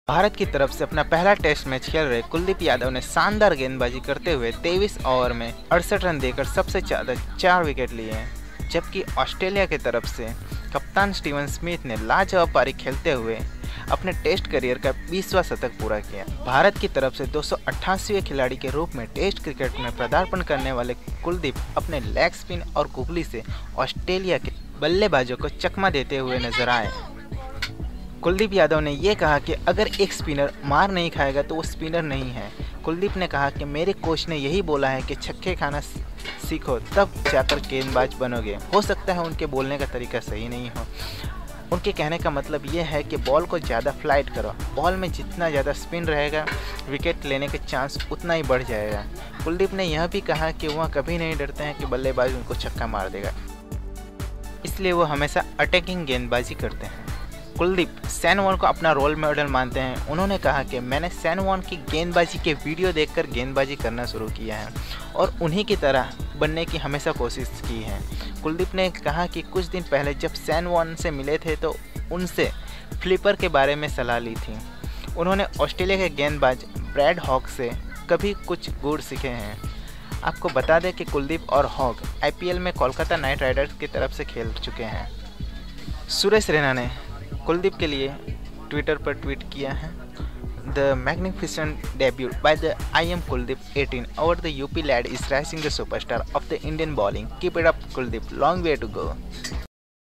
India v Australia: Joy after Kuldeep’s dream Test debut | Took 4 wickets in a Debut
The whole family was glued to the TV, when Kuldeep took his first wicket. Mother Usha, father Ram and sister couldn't believe it. What to do? Tears rolled down their eyes, but soon celebrations started and they treated each other with sweets.
When second wicket fell, there was loud celebration in the house. The neighbours joined the party as soon as Kuldeep took his third wicket. Now Ram Singh started getting calls from their relatives. A proud Ram couldn't hide the happiness.